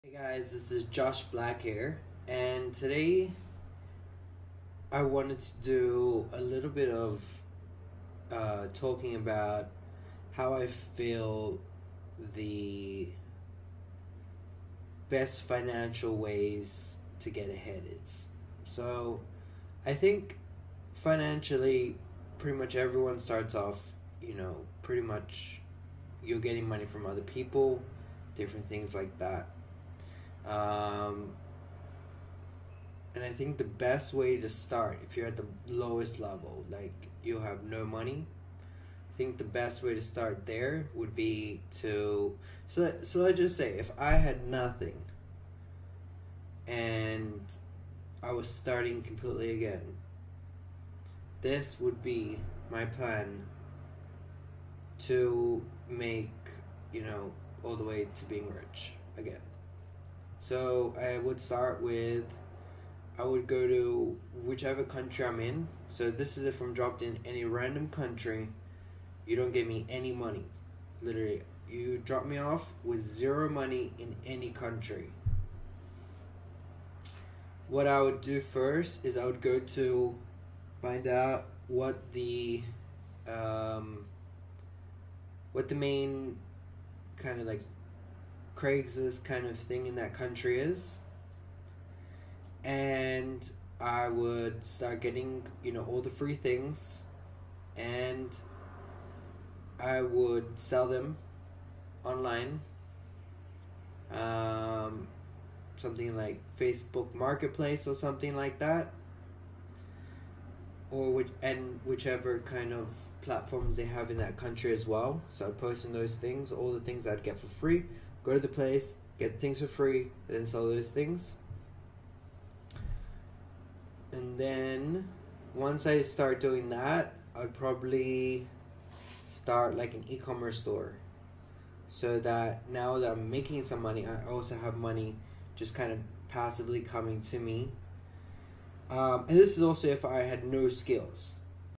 How to Get Rich Starting from Nothing My Guide to Financial Success
Hey guys this is Josh Gold here and today I wanted to do a little bit of talking about how I feel the best financial ways to get ahead is So I think financially pretty much everyone starts off you know pretty much you're getting money from other people different things like that And I think the best way to start if you're at the lowest level like you have no money I think the best way to start there would be to So let's just say if I had nothing and I was starting completely again this would be my plan to make you know all the way to being rich again so I would start with I would go to whichever country I'm in so this is if I'm dropped in any random country you don't give me any money literally you drop me off with 0 money in any country What I would do first is I would go to find out what the main Craigslist kind of thing in that country is and I would start getting all the free things and I would sell them online Something like Facebook Marketplace or something like that And whichever kind of platforms they have in that country as well So posting those things all the things I'd get for free Go to the place get things for free then sell those things And then once I start doing that I'd probably start like an e-commerce store So that now that I'm making some money I also have money just kind of passively coming to me And this is also if I had no skills